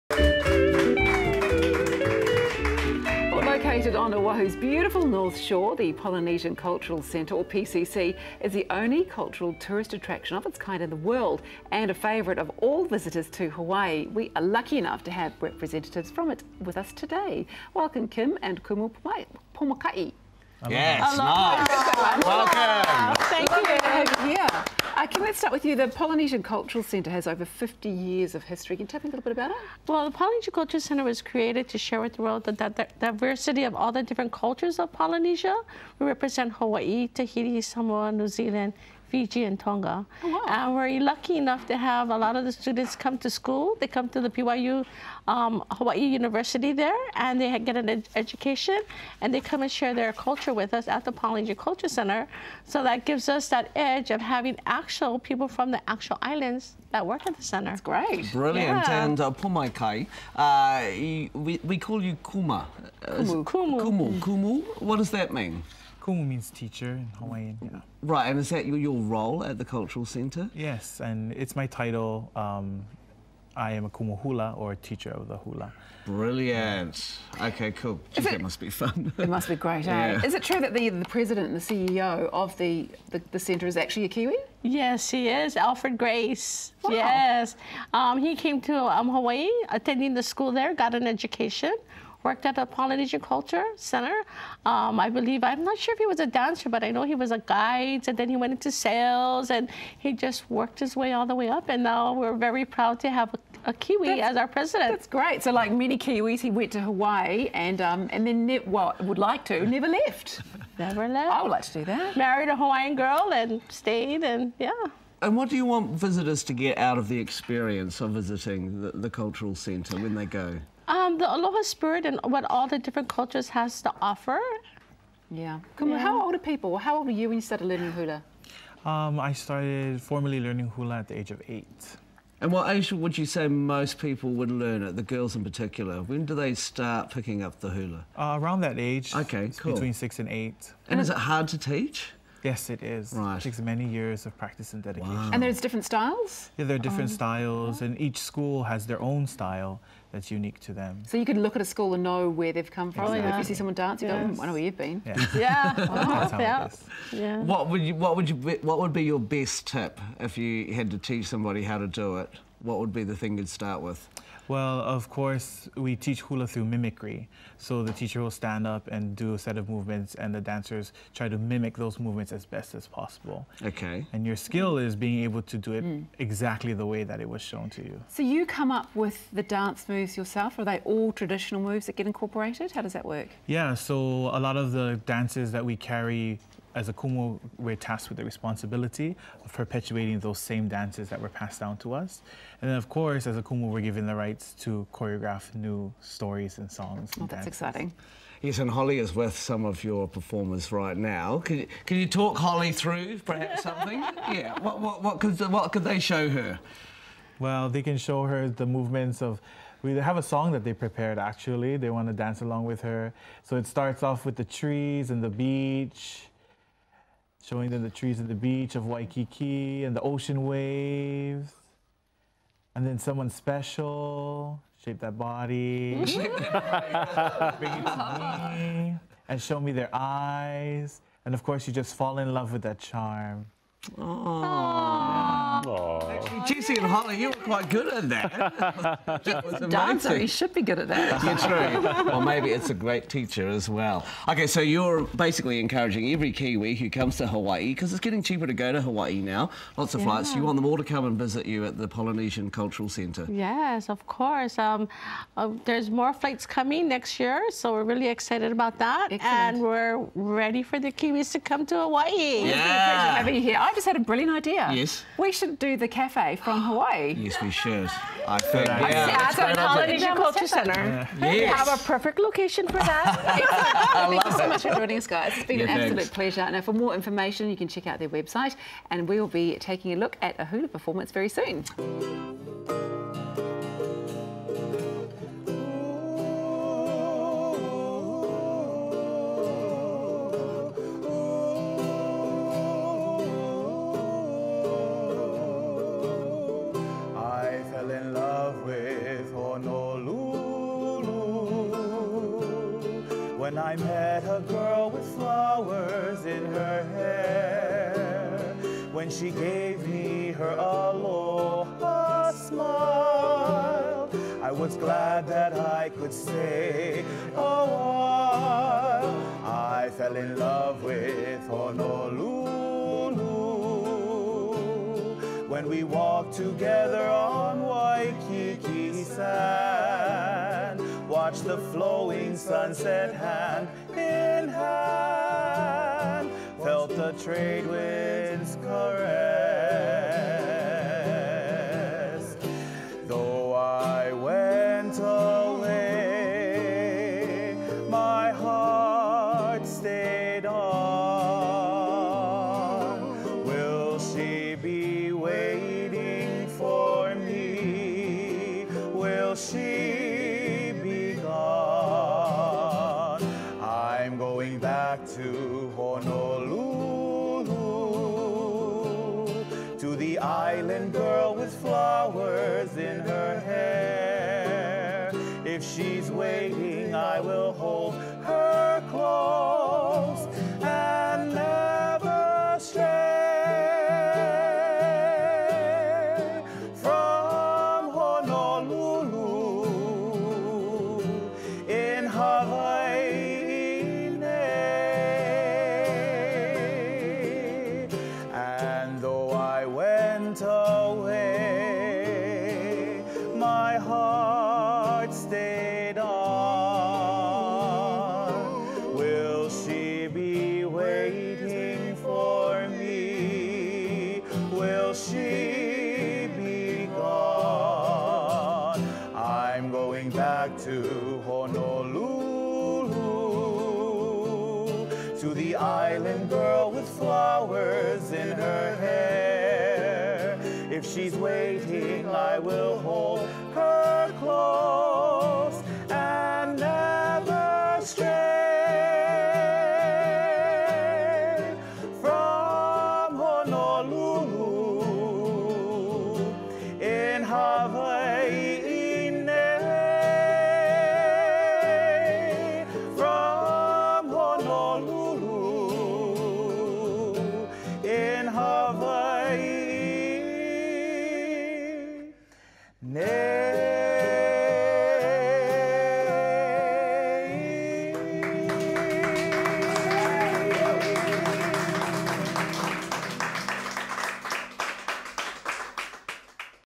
We chat about the Polynesian Cultural Center with Kim and Kuma!
Located on Oahu’s beautiful North Shore, the Polynesian Cultural Center (or PCC) is the only cultural tourist attraction of its kind in the world and a favourite of all visitors to Hawaii. We are lucky enough to have representatives from it joined us this morning on the show!